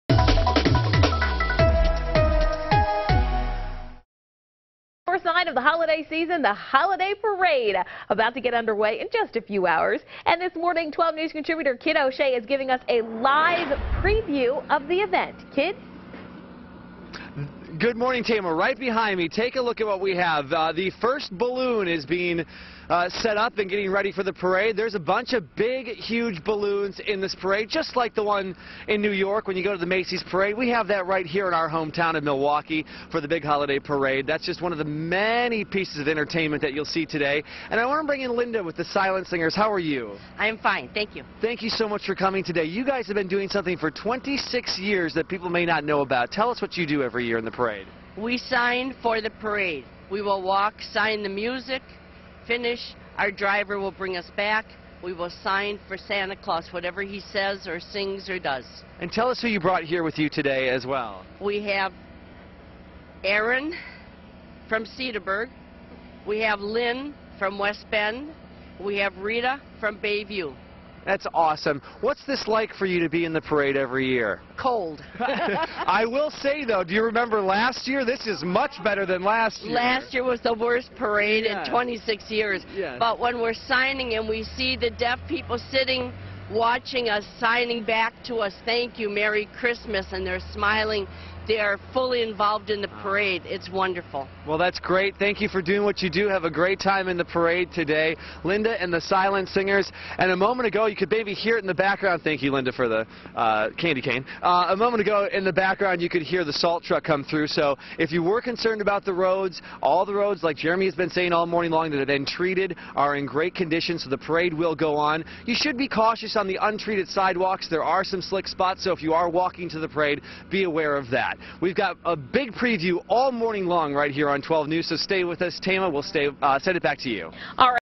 Preview of the Holiday Parade
12 News Contributor Kidd O'Shea gives us a preview of the Silent Singers, who will be apart of today's parade.